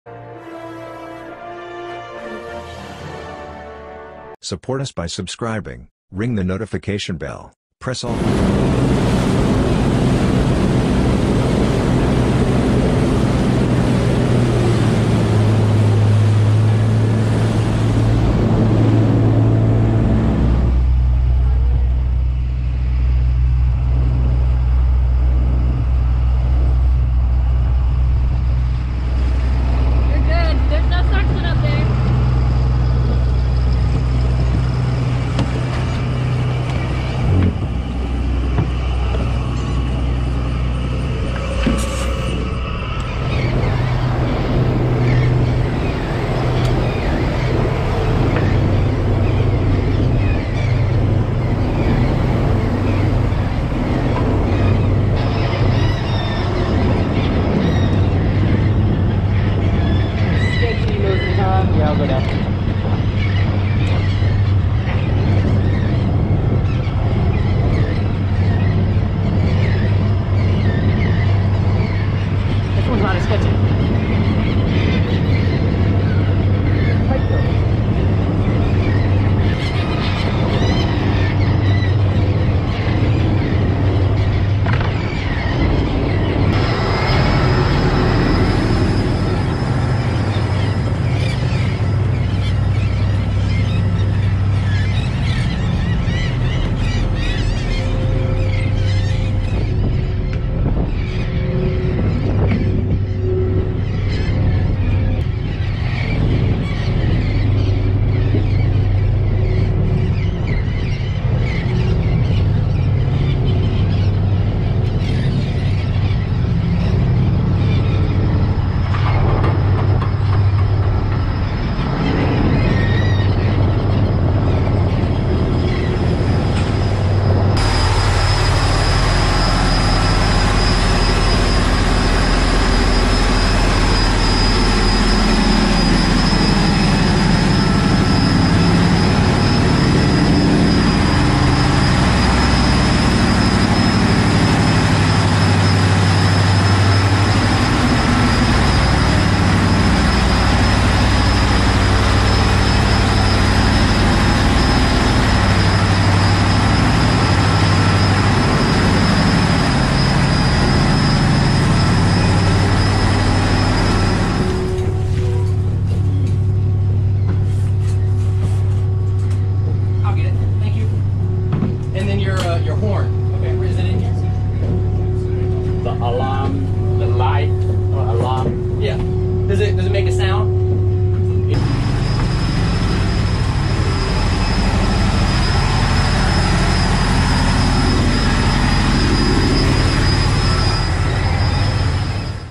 Coast Guard Boards Large Shrimp Boat for Inspection
Coast Guard Cutter Daniel Tarr law enforcement crew members conduct a fisheries boarding aboard a shrimping vessel off the coast of Louisiana. Fishery boardings are conducted to ensure commercial fisheries are following federal laws and regulations on the water.

vid credit to  Petty Officer 3rd Class Alejandro Rivera